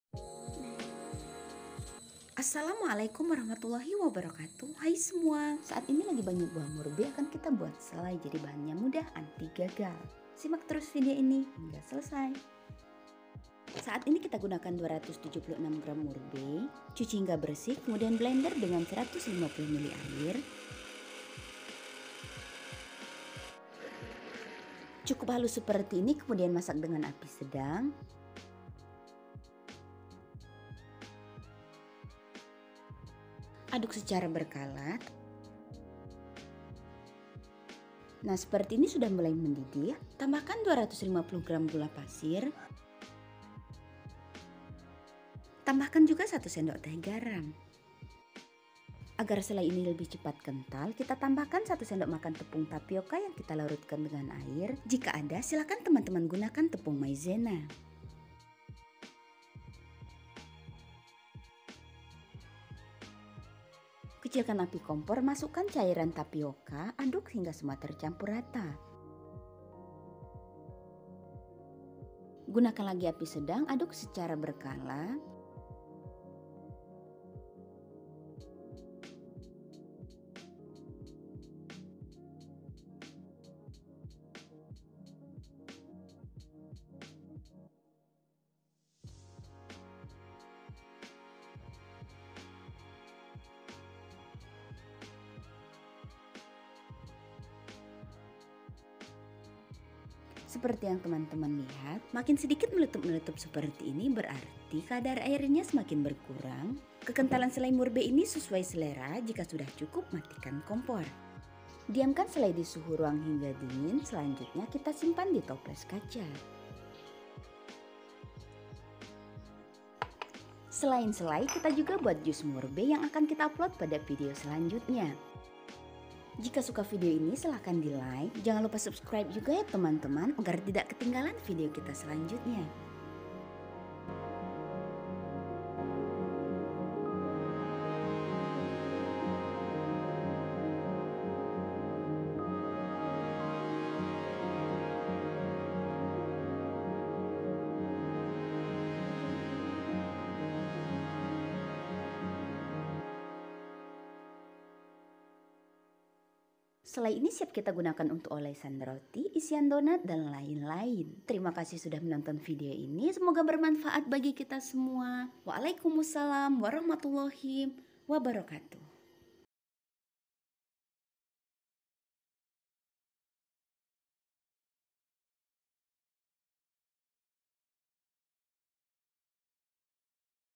Cara Membuat Selai Murbei || Mudah dan Anti Gagal
Dimusim hujan murbei berbuah lebat, dipanen buahnya lumayan banyak, kita buat selai murbei. Buatnya mudah dan anti gagal, adapun bahan bahan yang kita gunakan, yaitu:

BAHAN-BAHAN:
🍇276 gr      Buah Murbei

CARA MEMBUAT:
🍇 Cuci bersih murbei, blender dengan air
🍇 Masak dengan api sedang, jika sudah mulai mendidih masukkan gula dan garam
🍇 Setelah gula larut, selai murbei mulai menyusut, kecilkan api, masukkan larutan tapioka, langsung aduk hingga tercampur rata agar tidak menggumpal.
🍇 Gunakan lagi api sedang, masak selai hingga kadar air berkurang (ciri-cirinya meletup-letup pada selai berkurang dan seleai mulai mengental.
🍇 Setelah dirasa cukup kental, matikan kompor
🍇 Diamkan disuhu ruang, setelah dingin simpan di toples kaca.

TIPS:
🍇Tepung Tapioka dapat diganti dengan tepung Maizena
🍇 Lerutkan terlebih dahulu tepung tapioka agar lebih mudah tercampur.
🍇 Saat masukkan cairan tapioka, kecilkan api agar tidak menggumpal
🍇 Simpan selai dikulkas agar lebih awet.